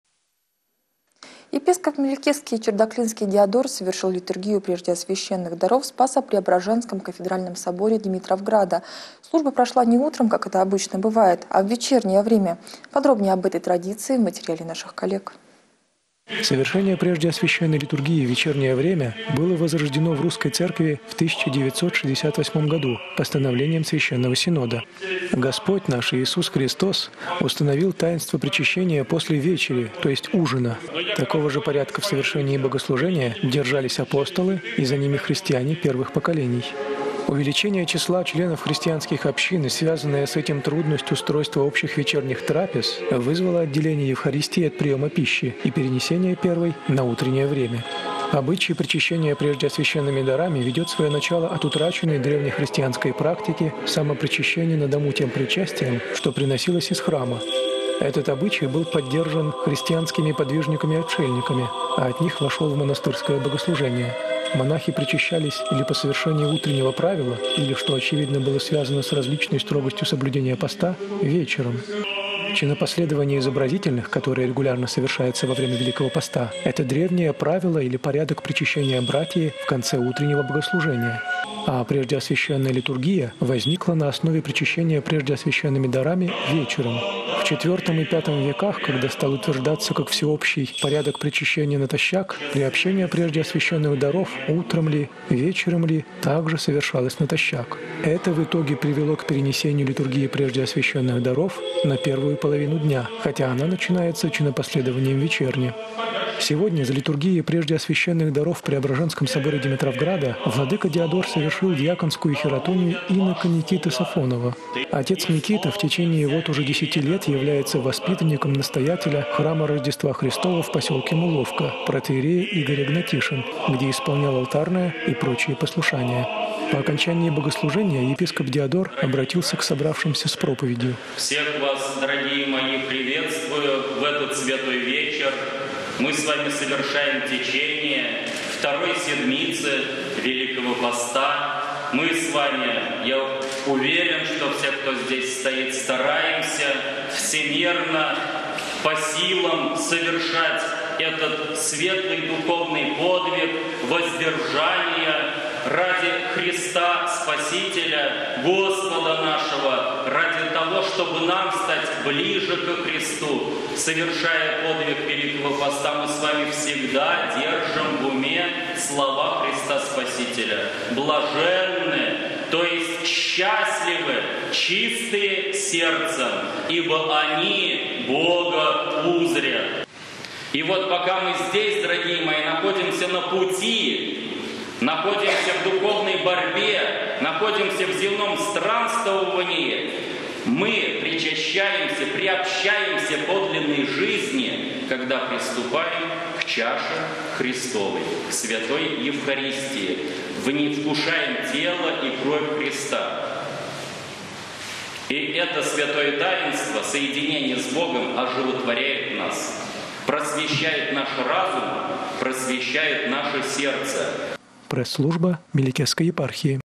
В Димитровграде возрождается древняя традиция вечернего служения Преждеосвященной литургии
Епископ Мелекесский и Чердаклинский Диодор совершил литургию преждеосвященных даров в спасо-преображенском кафедральном соборе Димитровграда. служба прошла не утром, как это обычно бывает.